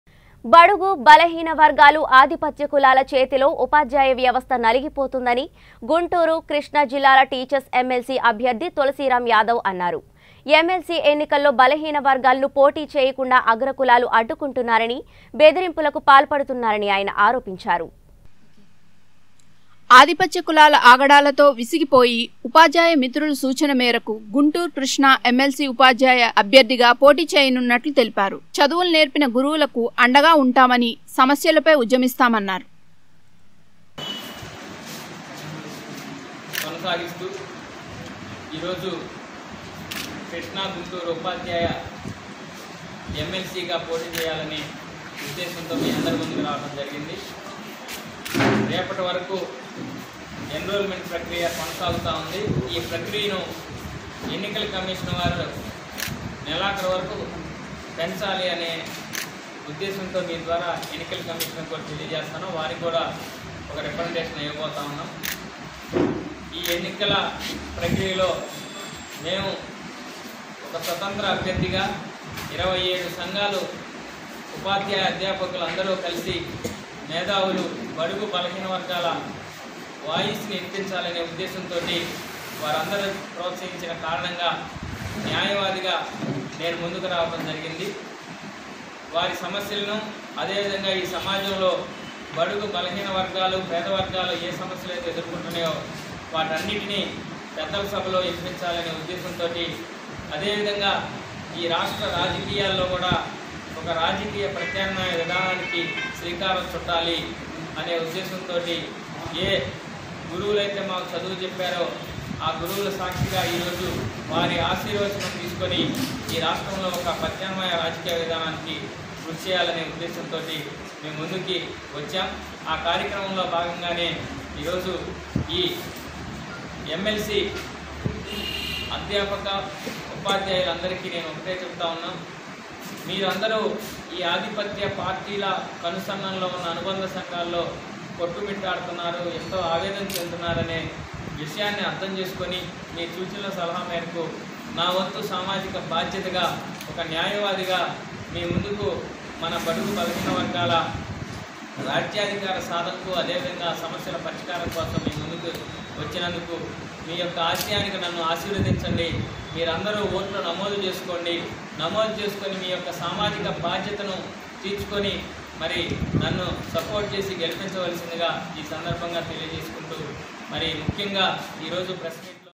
ఆధిపత్య కులాల చేతిలో ఉపాధ్యాయ వ్యవస్థ నలిగిపోతుంది | ఎమ్మెల్సీ అభ్యర్థి తులసీరామ్ | 19 Newstelugu
ఆధిపత్య కులాల చేతిలో ఉపాధ్యాయ వ్యవస్థ నలిగిపోతుంది | ఎమ్మెల్సీ అభ్యర్థి తులసీరామ్ | కృష్ణా జిల్లా  | 19 Newstelugu